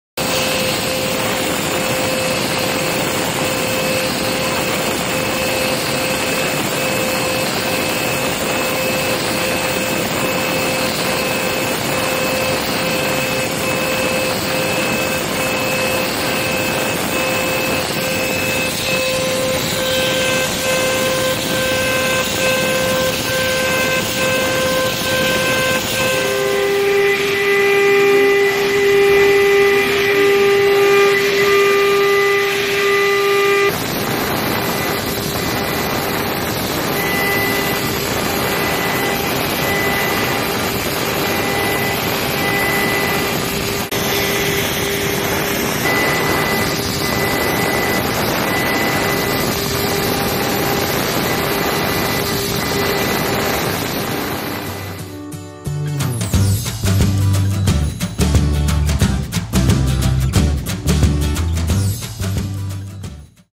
3D contouring milling / Profile machining with draft angle by Nine9 45 deg V insert
3D contouring milling / Profile machining with draft angle by Nine9 45 deg V insert.
Nine9 V insert tool (Engraving tool)  help you to obtain high speed and feed rate, dramatically reducing your cycle time.
widely be used on mold and die, machine components, medical components, gun components, automotive parts, gears, bearings and luxury goods.